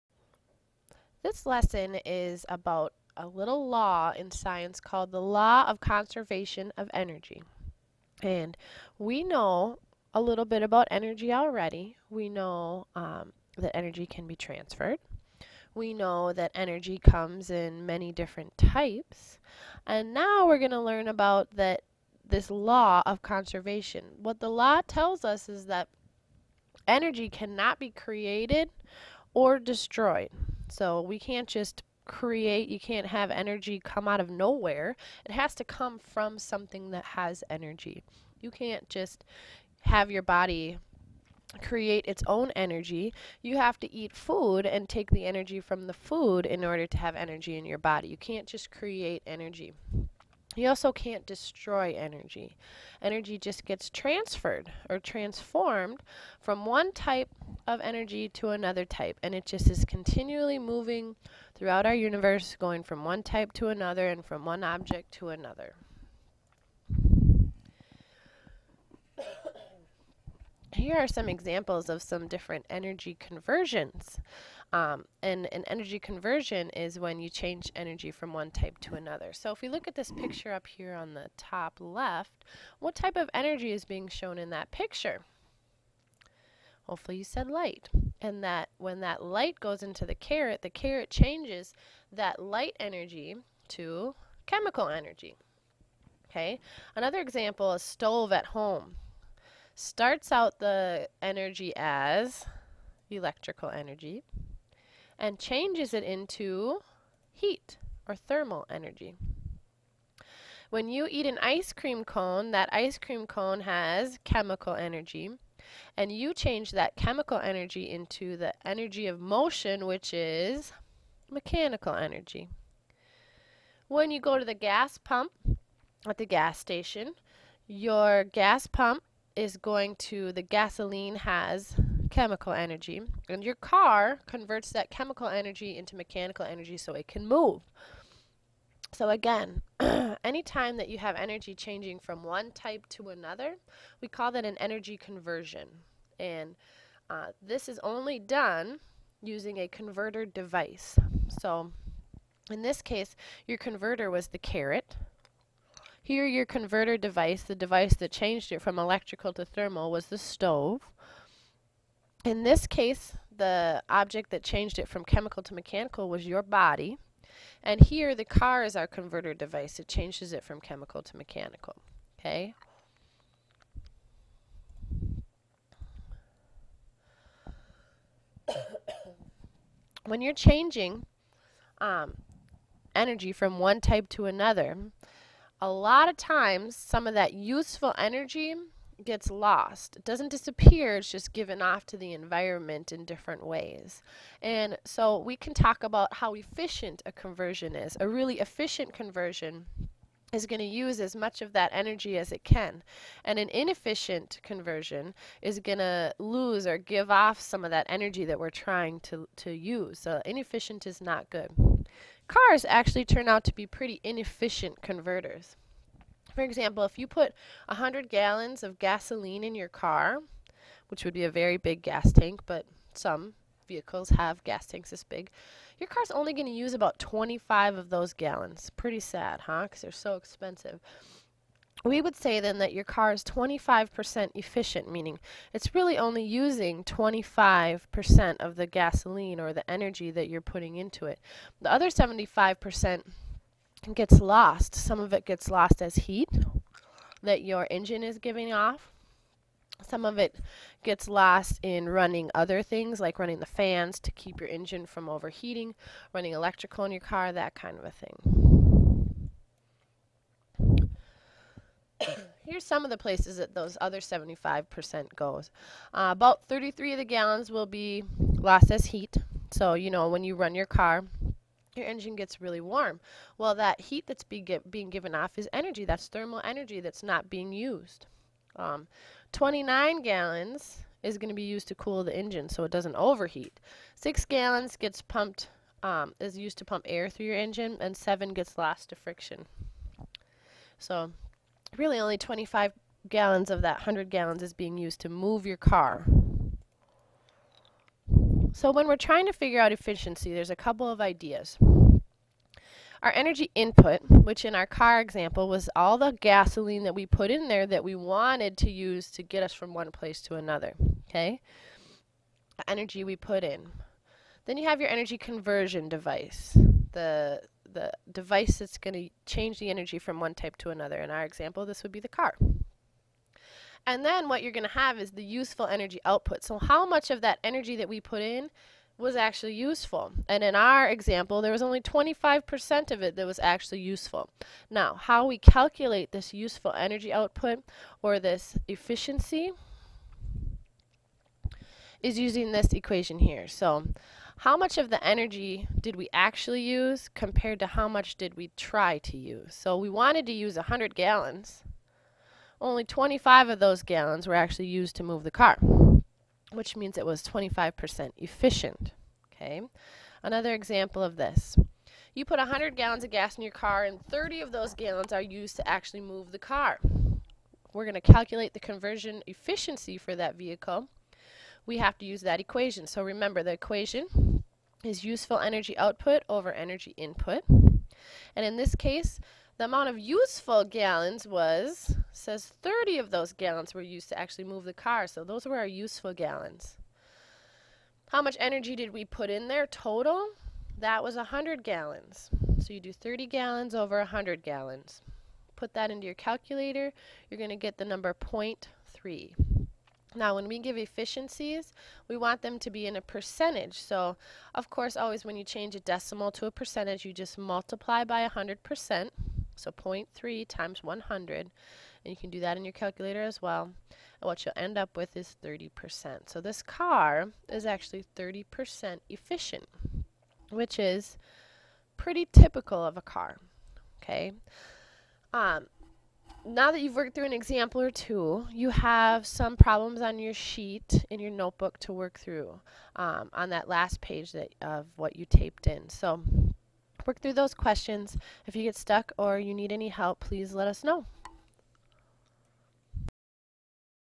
Conservation of Energy Notes
The Law of conservation of energy and conversion efficiency.